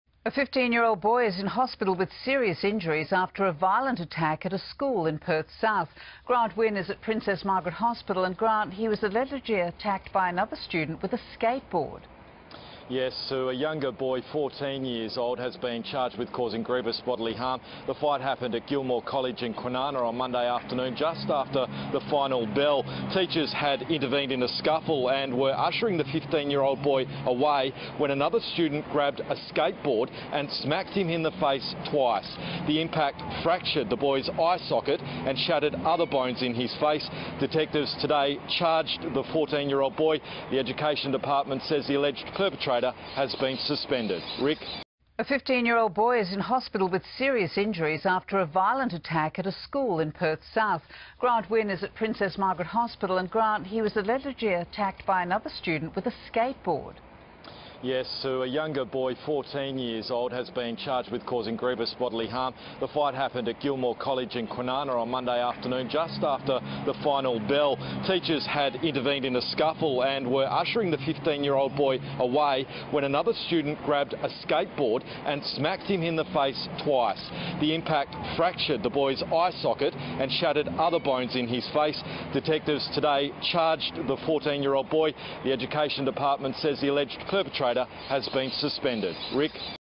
Boy hospitalised after attack at Perth school
A 15-year-old boy is in hospital with serious injuries after a violent attack at a school in Perth's south.